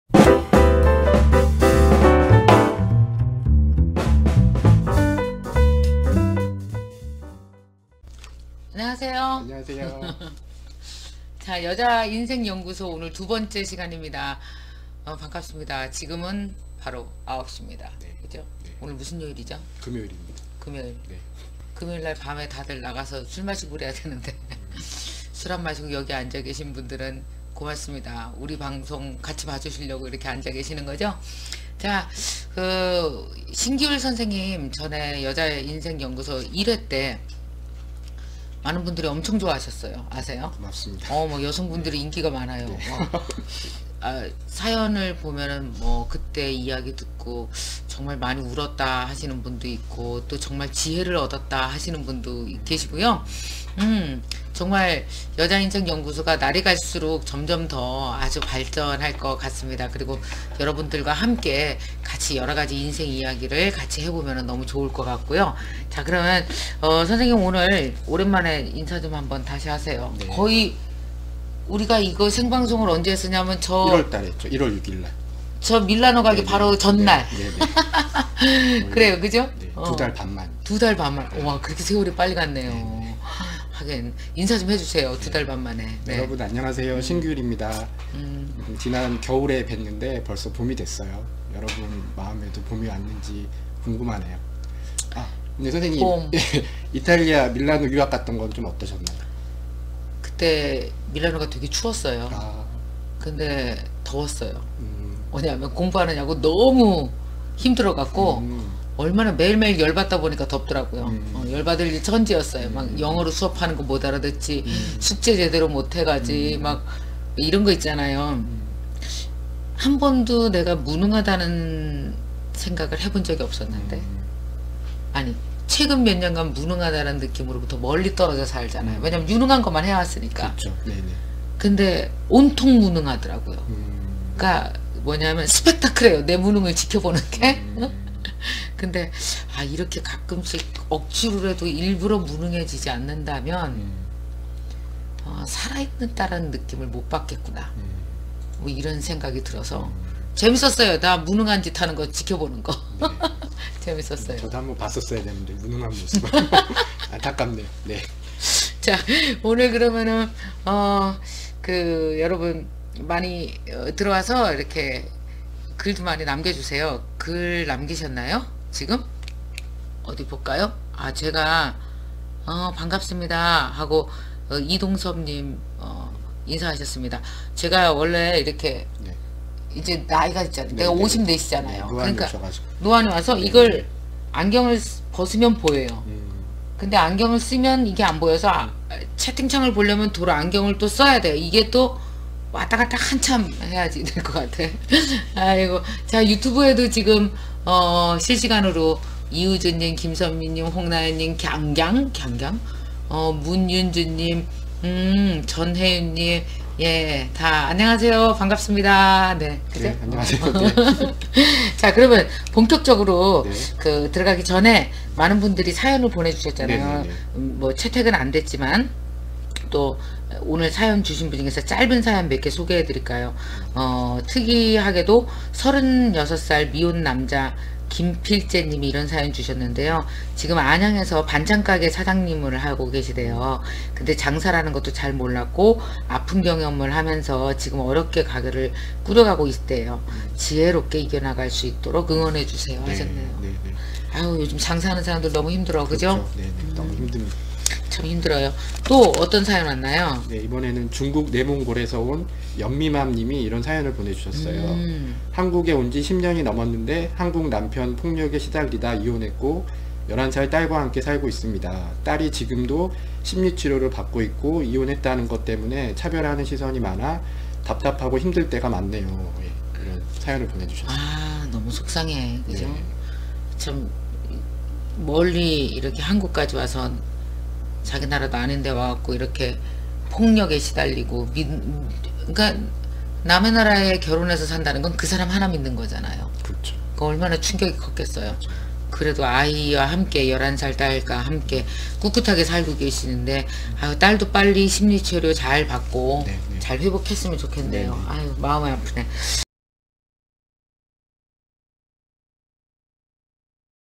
인간관계 어렵다면? #1 - 김미경 신기율의 여자인생연구소 '고민특집'
김미경 신기율의 여자인생연구소 고민특집 #1
[인간관계 어렵다면?]

Q:
처음에는 금방 말이 잘 통하고 쉽게 친해졌는데
시간이 지날수록 선을 넘고 무례한 사람들이 많아요.

그래서 스트레스 받다가 조금씩 선을 긋다 보니
결국 주변에 사람이 없어서 외롭고 우울해집니다

A:
이런 고민을 하시는 분들이 의외로 참 많은데요.
이럴때는 어떻게 하면 좋을까요?

다른 데는 문제가 없는데 유독 대인관계에만 고민이 있다면
‘고민을 다루는 방법’을 바꿔보는 것이 좋습니다.

고민을 한가지에 너무 깊게 집중하지 말고
자잘한 고민들로 분산시켜 버리는 것이지요

그렇게 균형을 잡기 시작하면 너무나 크게 보였던 고민이
나도 모르게 작아져 있음을 발견하게 됩니다.

▶스타강사 김미경 방송을 놓치지 않고 모두 보려면 ‘구독’, ‘좋아요’ 클릭!